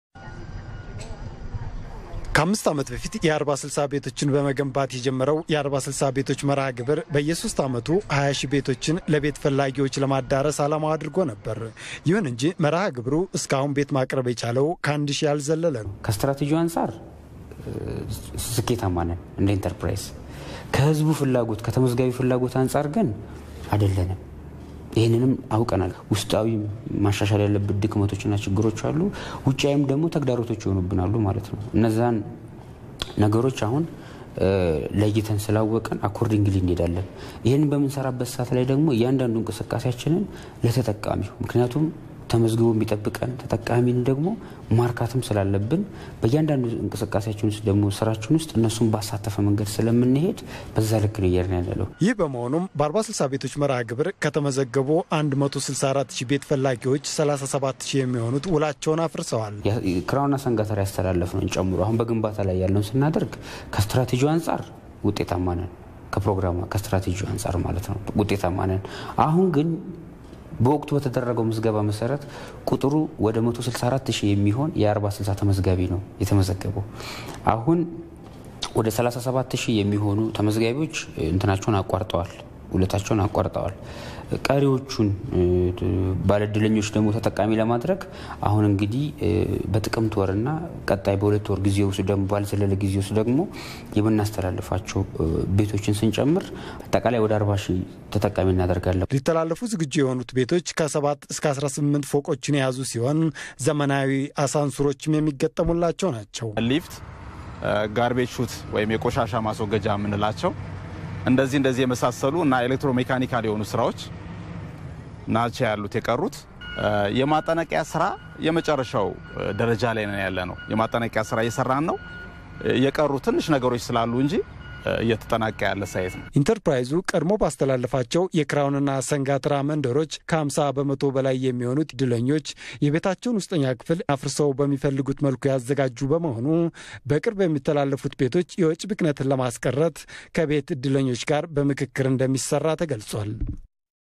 lates news hope about *40/60 housing project
164,000 registered 5years ago 37,000 dropped out and now 17,000 housesto be delivered in two months time. Who will be lucky??